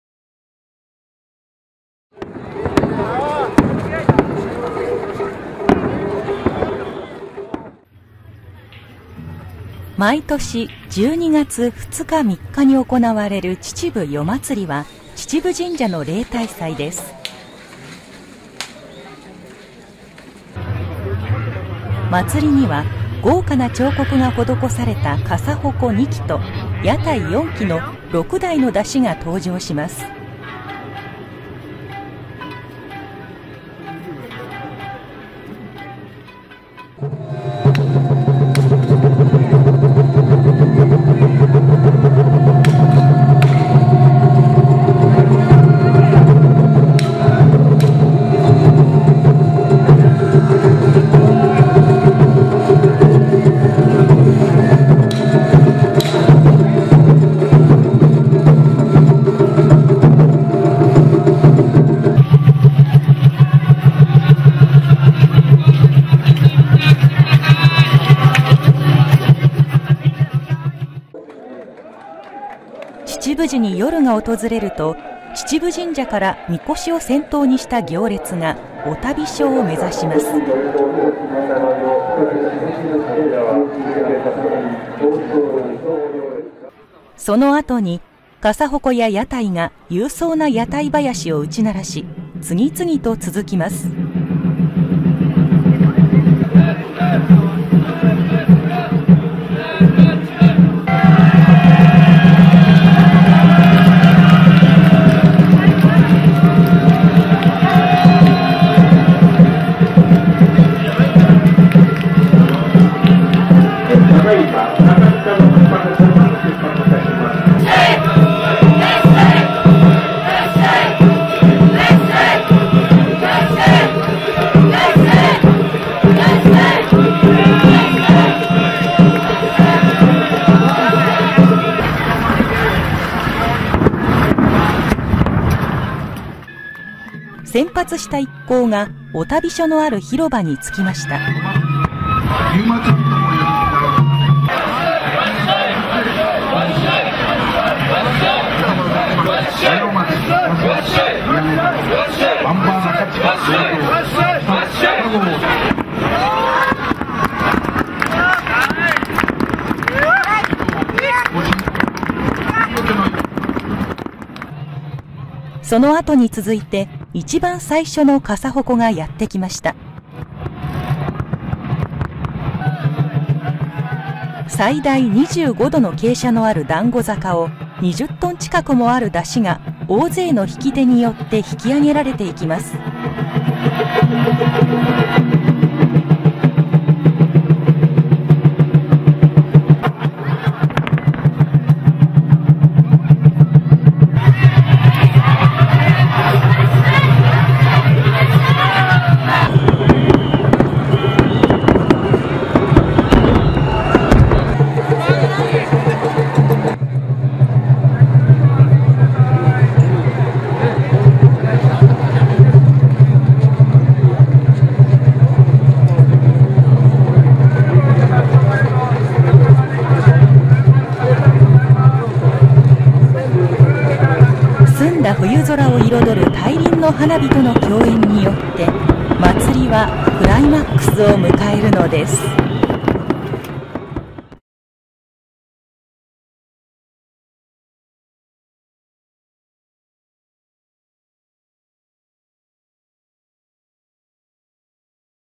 秩父夜祭（简体）【埼玉県公式観光動画】
秩父市的腊月有名的东西，秩父神社的例大祭「秩父夜祭」，以300多年的历史而自豪，与京都只园祭，飞騨高山节一起被日本三大曳山节的1个数。祭礼当日绚烂豪华的2台斗笠戟和4台货摊子被曳引，向(到)冬天清澈的天空各种各样的烟火能鲜明地发射。

咨询处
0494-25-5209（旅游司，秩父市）
旅游及秩父